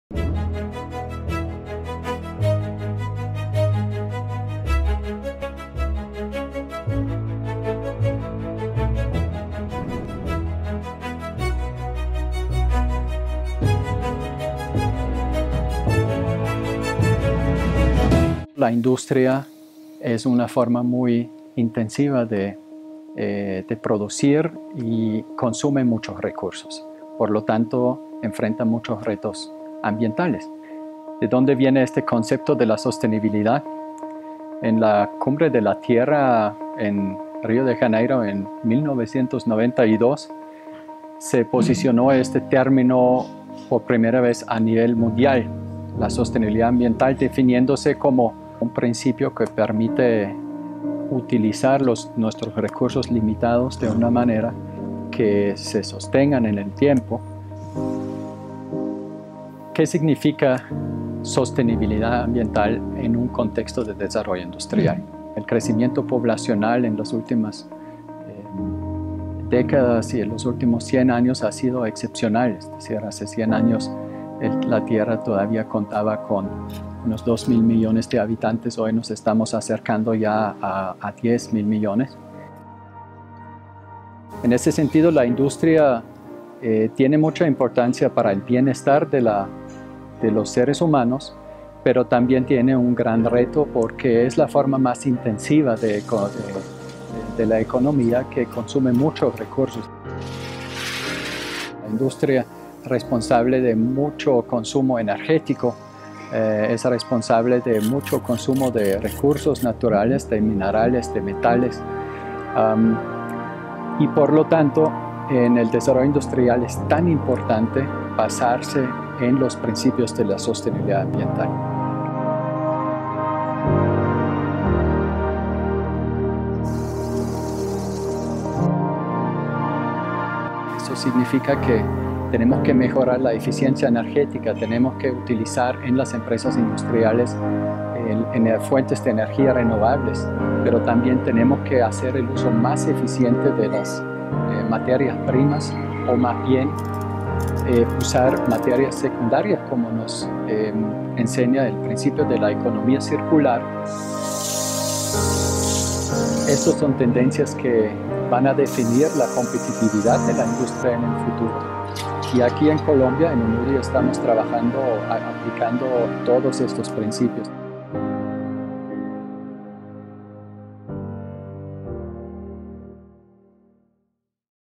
Capítulo 1: Sostenibilidad Ambiental
¡Bienvenidos!

Este es el primer capítulo de "Construyendo Industria Sostenible", la primera serie web de ONUDI Colombia que aborda la sostenibilidad en un contexto industrial.

Nuestro mandato es promover un desarollo indutrial sostenible y lo hacemos aplicando los  Principios de Sostenibilidad Ambiental.

En Colombia, con el acompañamiento a empresas para reducir su consumo energético a través de la implementación de  Sistemas de Gestión Energética, se ha logrado un ahorro de hasta el 20% de su consumo energético.

ONUDI Colombia también apoya al Gobierno Nacional para lograr la eliminación del uso de mercurio en las economías locales bajo el marco del Convenio de Minamata. Además a través del Convenio se busca, no solo erradicar el uso del mercurio, sino permitirle a las comunidades que han practicado la minería informal, principal fuente de contaminación por mercurio desde hace mucho tiempo, practicarla de una manera sostenible.

Por otro lado, está la iniciativa de la "Química Verde", tendencia mundial hacia la conversión de todo el sector de la Industria Química hacia las materias orgánicas. En Colombia ya hay empresas que han dado el paso y por ejemplo, en vez de usar solventes tradicionales usan solventes hechos de residuos de frutas cítricas como el caso de la naranja que al introducir su residuo en un proceso industrial se convierte en una materia de mucho valor agregado y fuente de ingreso, permitiendo un estilo de economía mas sostenible en el tiempo y ofreciendo más oportunidades para los jóvenes en el futuro.